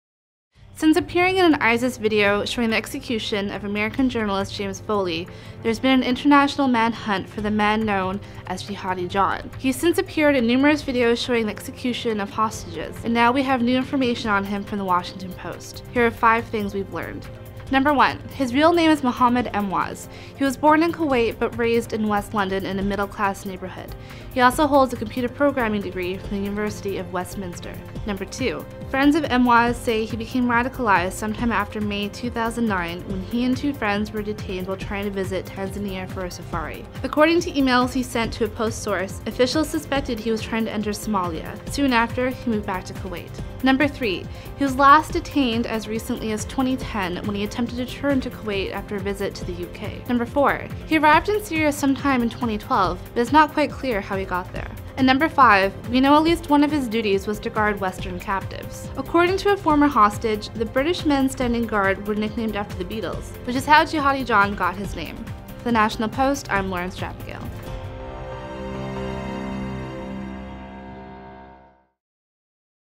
NP Explainer: Who is Jihadi John?
Lauren Strapagiel reveals new intel on Mohammed Emwazi, the alleged ISIS member responsible for the executions of several western reporters.